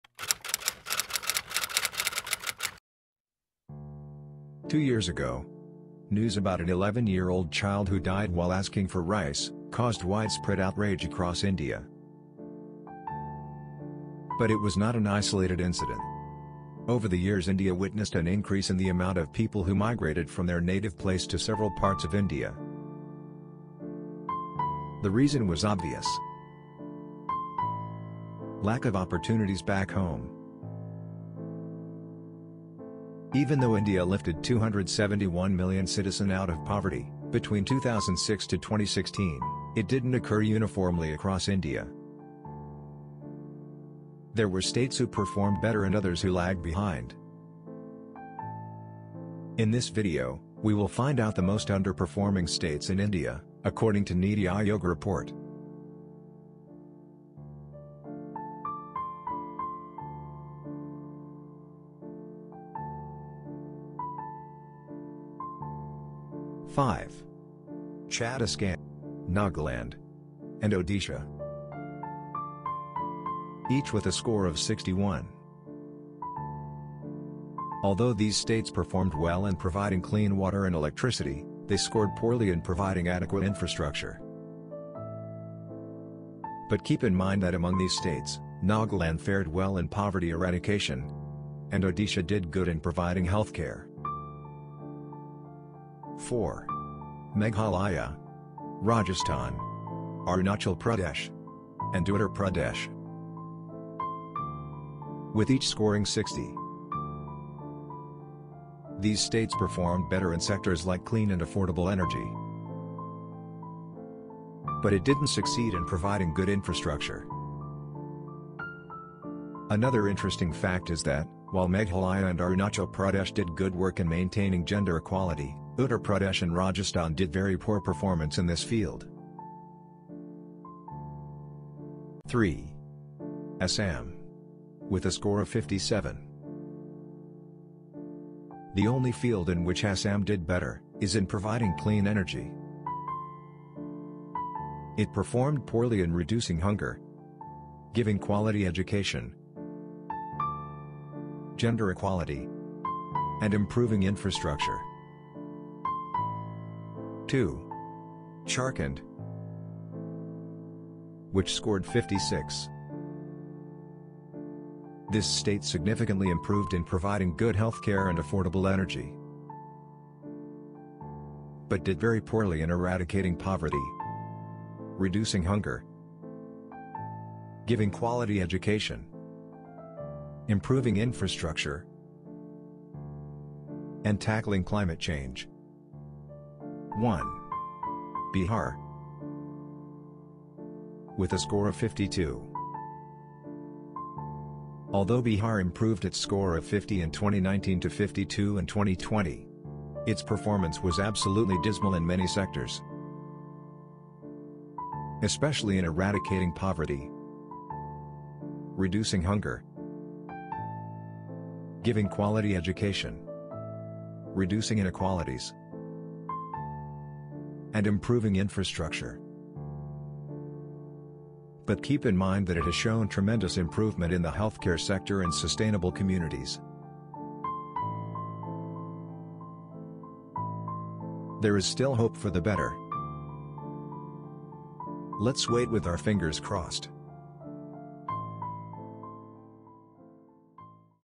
Worst performing states in India- 2021| Niti Aayog report
India is a very diverse country comprising of 28 states. In this video we will find out the bottom 5 worst performing states in India according to  SDG India Index by NITI Aayog.
It is based on the progress on the outcomes of  various schemes taken by the government.